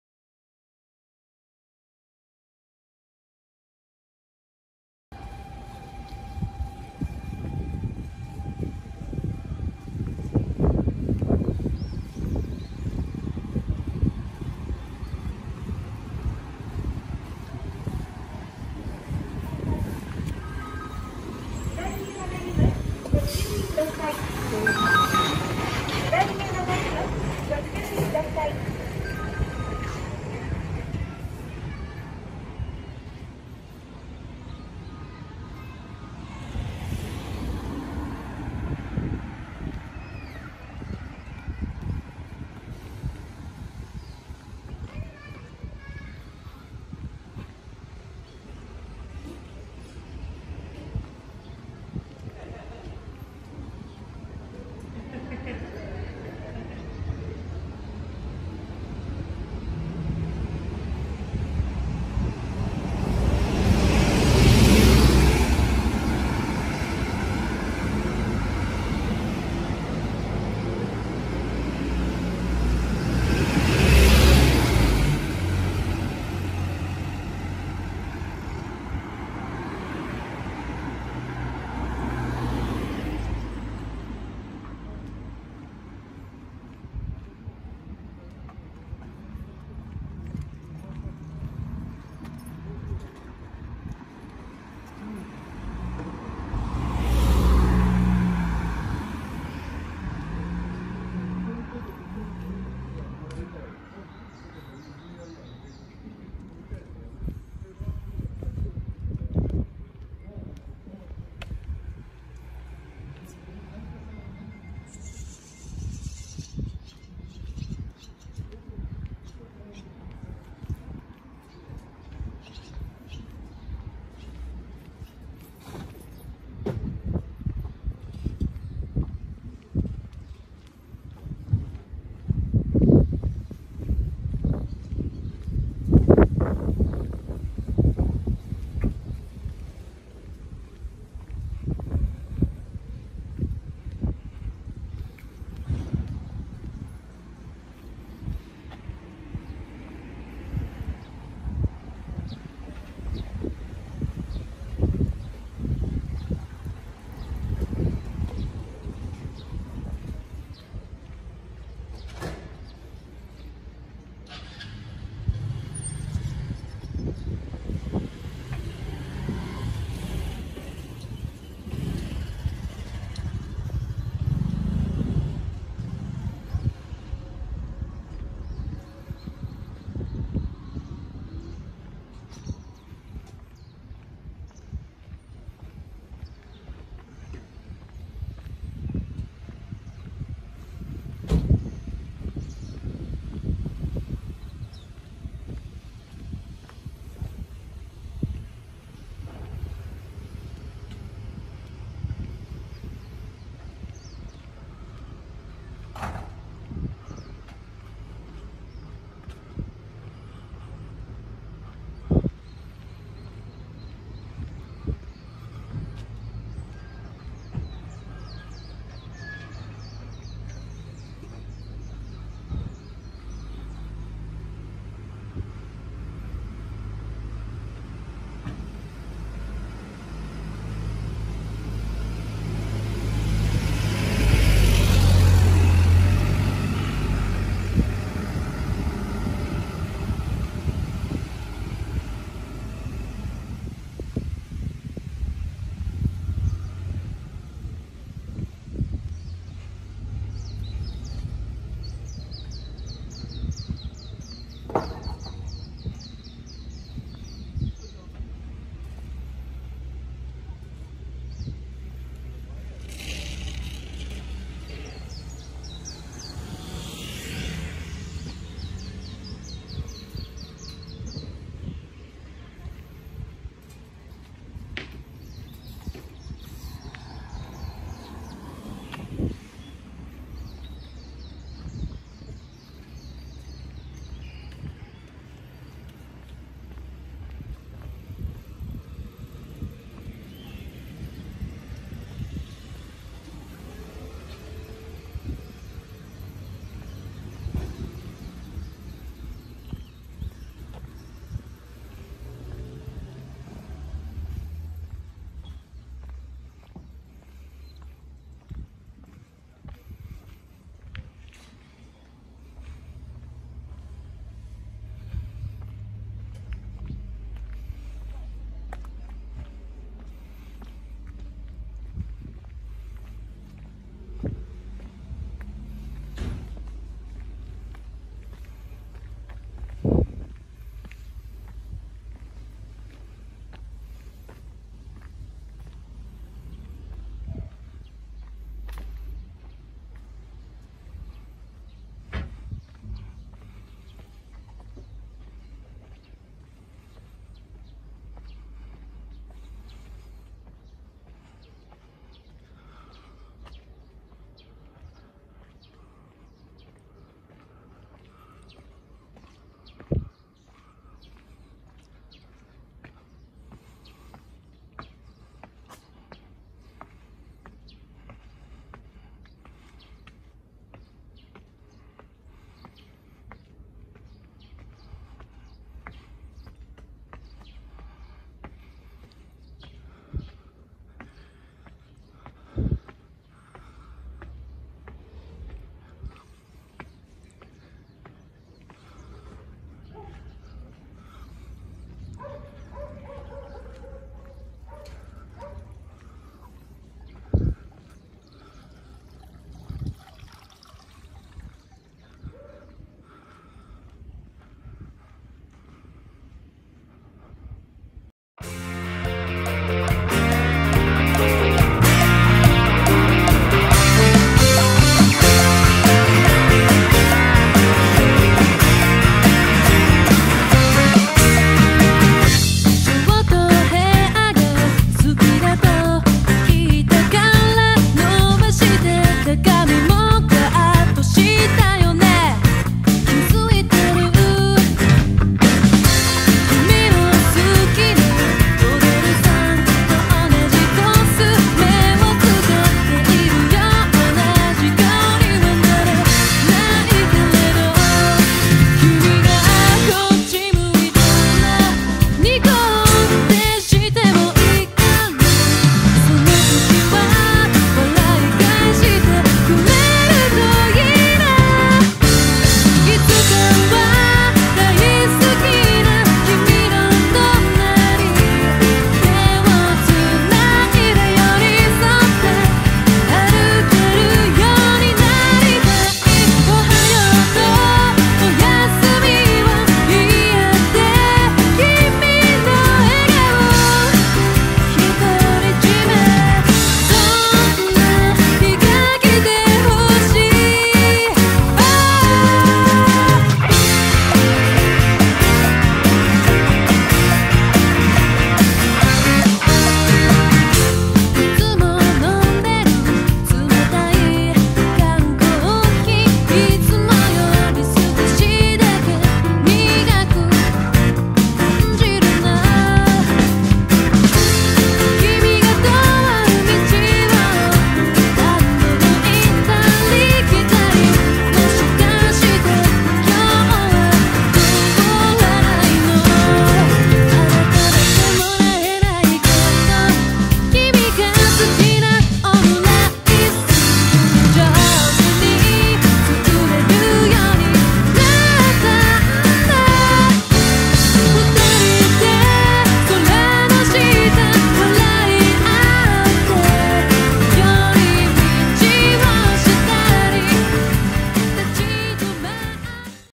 【西宮市地域情報動画】今年もバラが見頃！甲陽園駅からアンネのバラの教会への行き方
阪急甲陽園駅からアンネのバラの教会への順路を説明している動画です。動画の最後にはバラの写真もアップしています。byだぁ＠にしつー（奥大輔）

エンディングには西宮出身のシンガーソングライター梅原怜子さんの楽曲を使用しています。

今後も西宮に関する動画を色々と発信していく予定ですので、